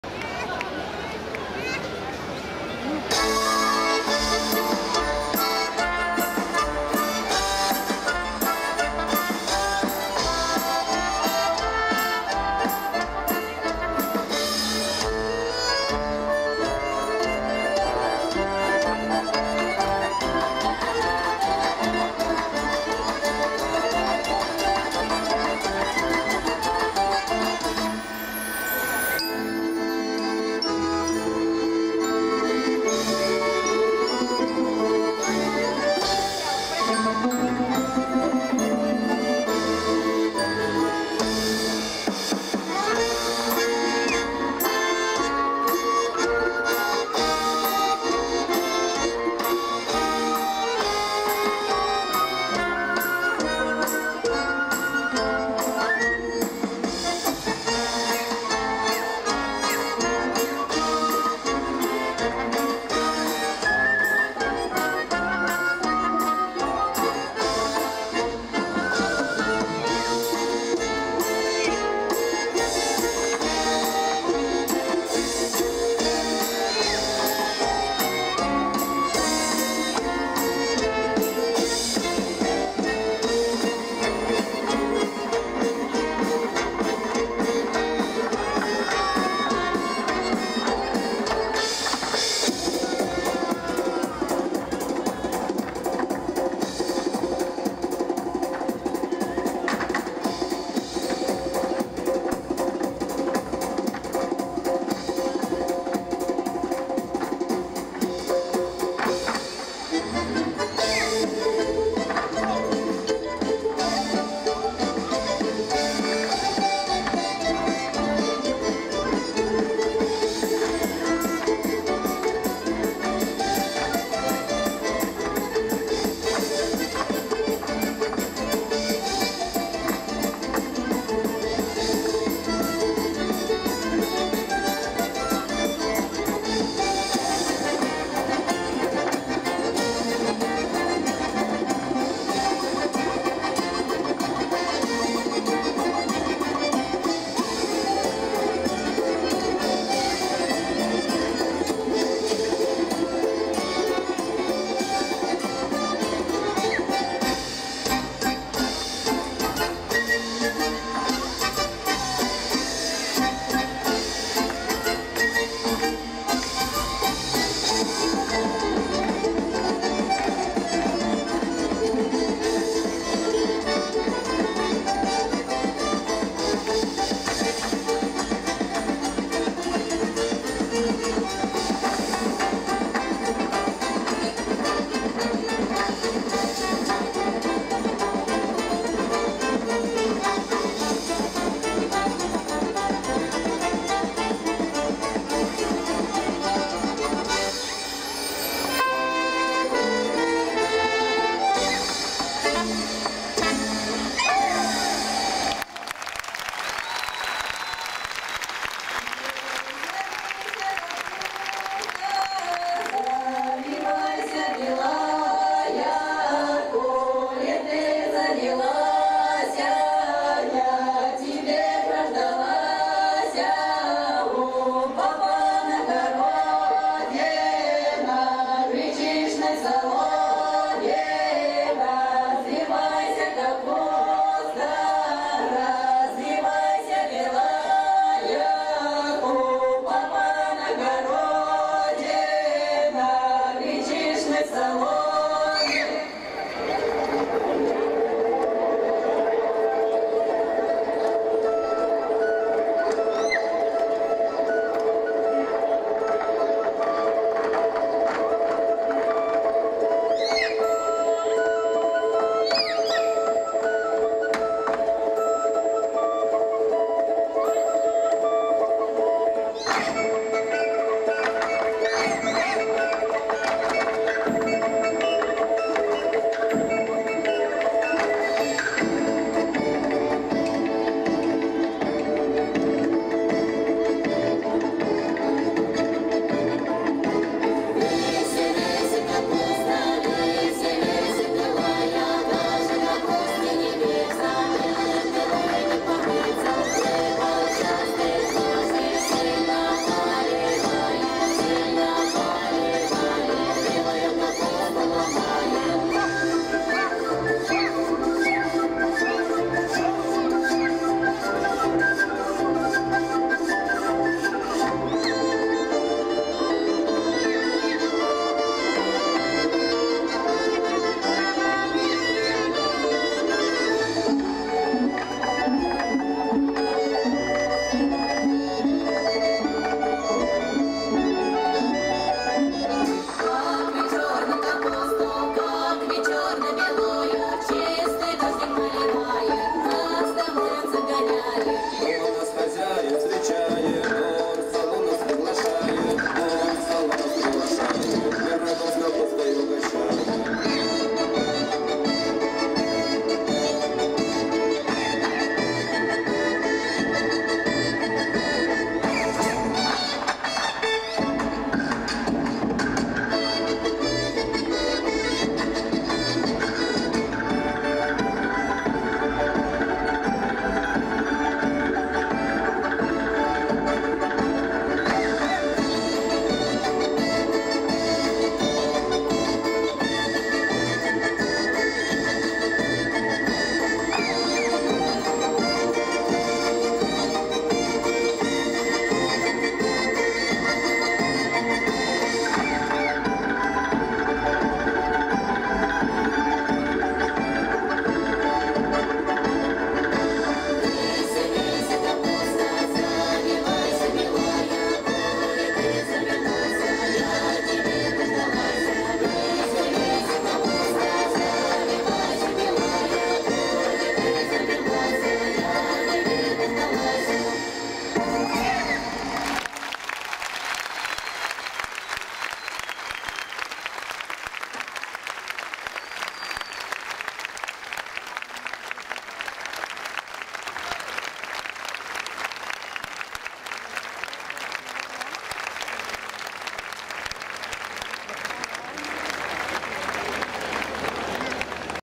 Russian folk dance: Kazachiya & Kapusta
Interpretation of a folk dance that originates from Russia. Done by Agricultural Academy Dance and Song Ensemble Razdolie, based in Buriatia, Russia. Recorded in Monção, on 31/07/2018.

Baile ruso interpretado polo Agricultural Academy Dance and Song Ensemble Razdolie, de Buriacia, Rusia. Gravado o 31/07/2018 en Monção, durante o Festival de Folclore Internacional Folkmonção O mundo a dançar.